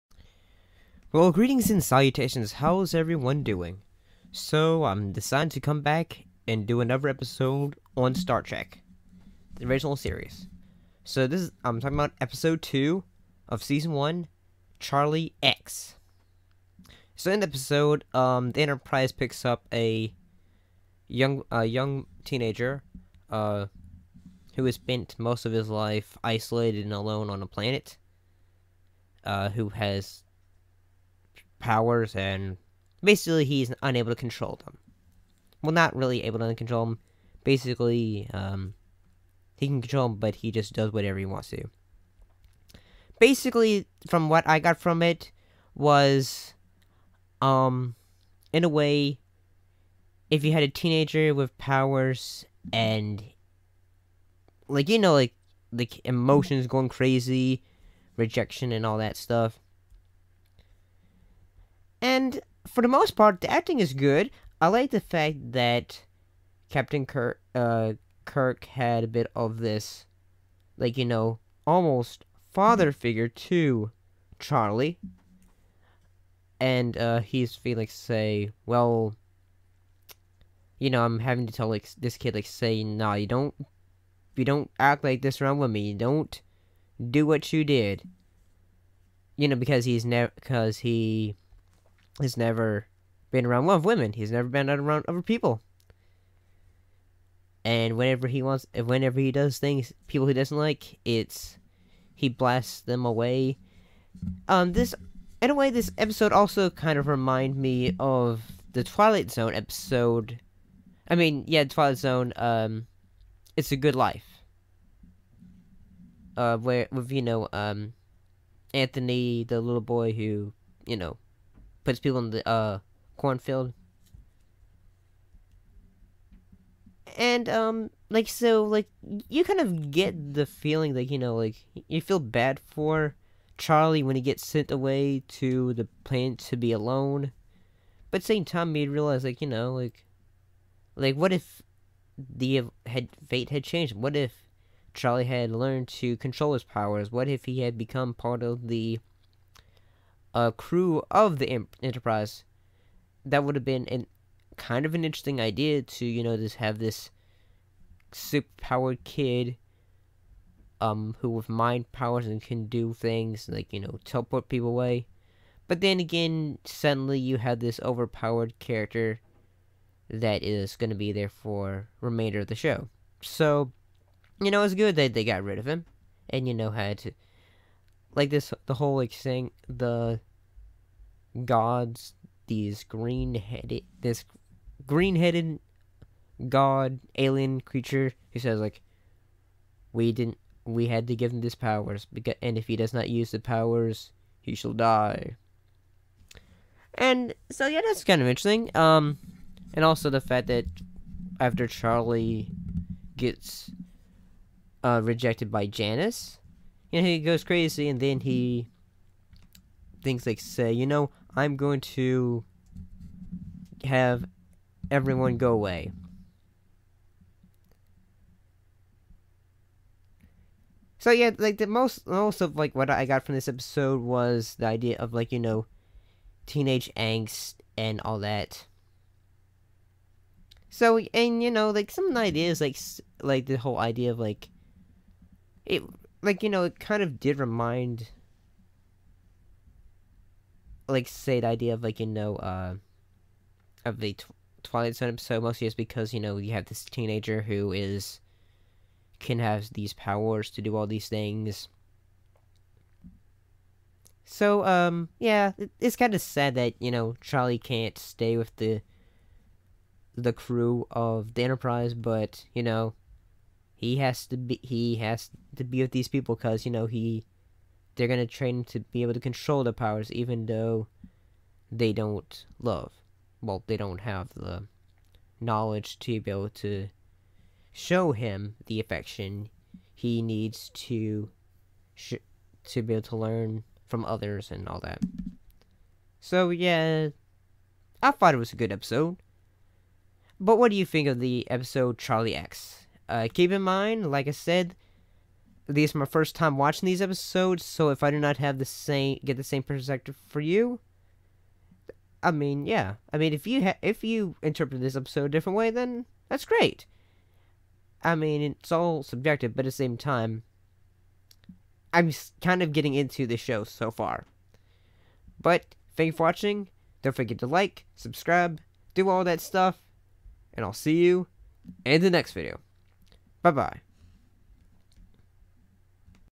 Star Trek S1 Ep2 Charlie X thoughts
First Thoughts after seeing Episode 2 Charlie X of the original Star Trek.